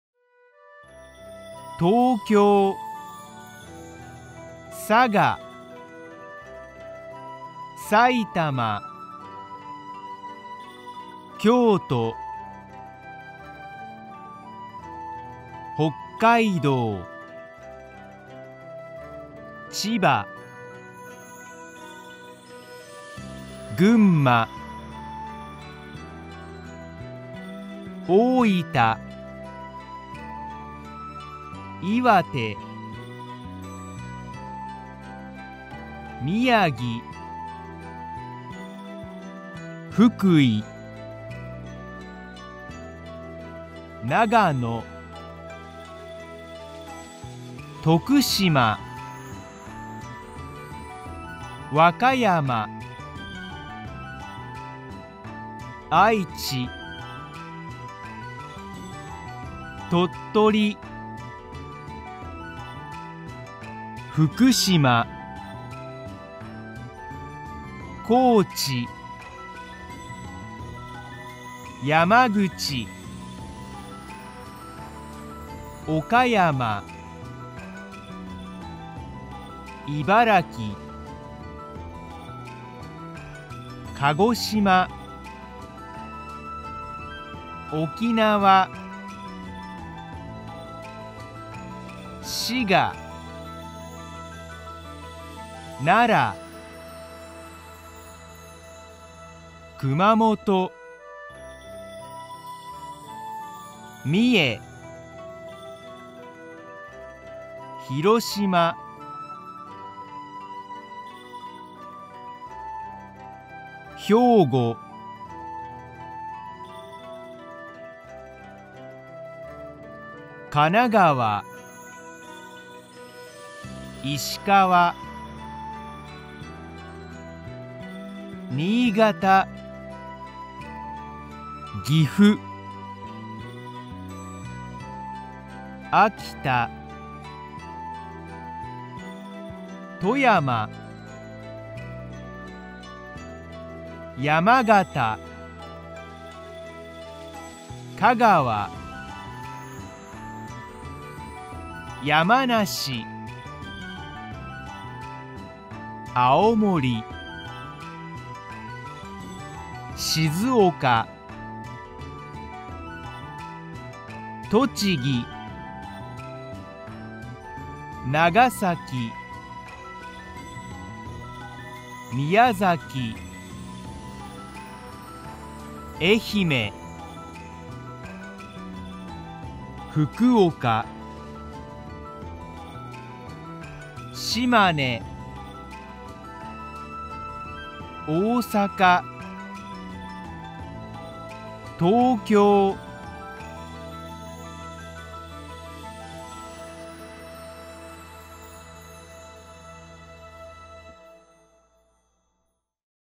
Japanese Prefectures Flags Animation - The Animated Flags of Japan
Flags of all 47 Japanese Prefectures, Animation with the pronunciation of the prefectures of Japan. This video is a collaboration with the animation artist Andrew Scelta.

This video includes the animated flag of every prefecture in Japan. The flag animation is accompanied by the pronunciation of each prefecture with the correct Japanese accent. I have added a piece of calming music from Japan to make the video more entertaining for the viewer.

Historically, Japan was divided into parts of the country controlled by Shogunate and feudal domains. This changed in the mid-19th century, when a new system was devised to classify administrative regions: the prefectures. Prefectures of Japan (都道府県, todōfuken) are one of the basic local entities of Japan. Prefectures are the governmental bodies of Japan which are larger than cities, towns, and villages. Japan is divided into a total of 47 prefectures; 43 proper prefectures, the 2 urban prefectures of Osaka and Kyoto, one ‘circuit’ or ‘territory’ (Hokkaido), and one ‘metropolis’ prefecture, Tokyo. Each prefecture is its own administrative jurisdiction and led by a directly-elected governor. A separate assembly decides separate budgets and ordinances for each prefecture, such as organizing the local police force and the supervision of local schools and hospitals.

The 47 prefectures of Japan in this video shown in order of appearance: Tokyo, Saga, Saitama, Kyoto, Hokkaido, Chiba, Gunma, Oita, Iwate, Miyagi, Fukui, Nagano, Tokushima, Wakayama, Aichi, Tottori, Fukushima, Kochi, Yamaguchi, Okayama, Ibaraki, Kagoshima, Okinawa, Shiga, Nara, Kumamoto, Mie, Hiroshima, Hyogo, Kanagawa, Ishikawa, Niigata, Gifu, Akita, Toyama, Yamagata, Kagawa, Yamanashi, Aomori, Shizuoka, Tochigi, Nagasaki, Miyazaki, Ehime, Fukuoka, Shimane, Osaka, and Tokyo.

Kiddopedia channel is the right place to find educational videos about countries, flags, animals, objects and English vocabulary.